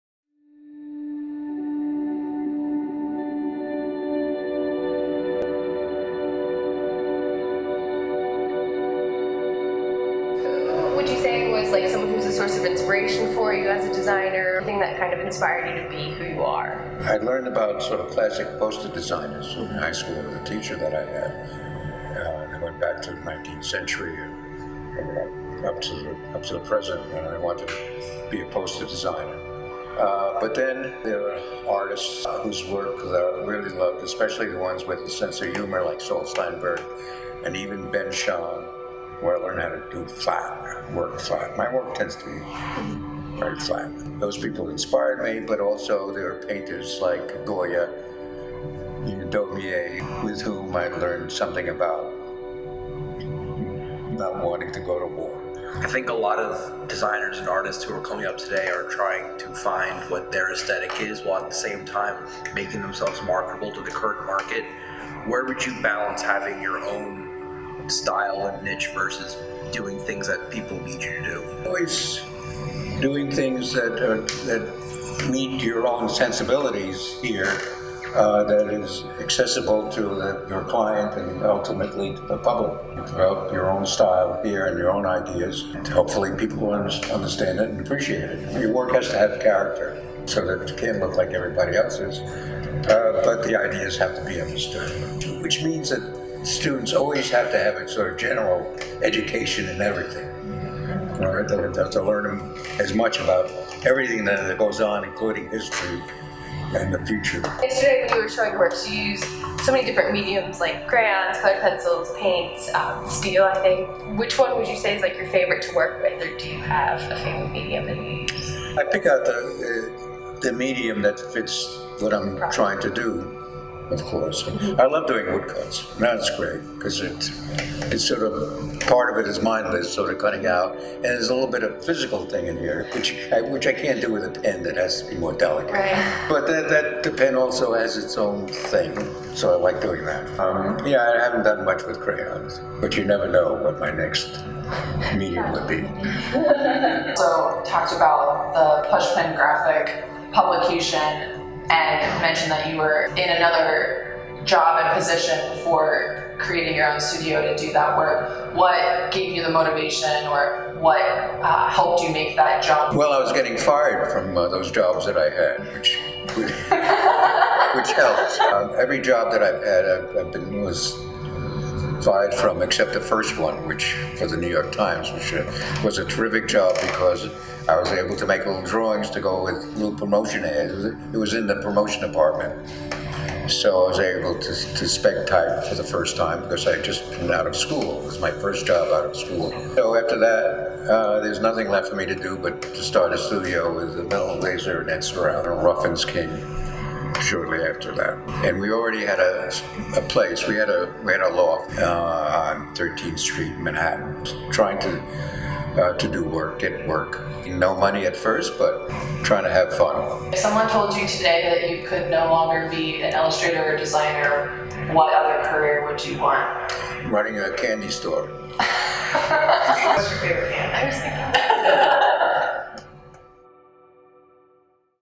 Designer Seymour Chwast at Moravian College
Students from the Moravian College Art Department had a chance to sit down with world-renowned designer Seymour Chwast during his visit to campus last week.

Follow Moravian College:

On Snapchat…moraviancollege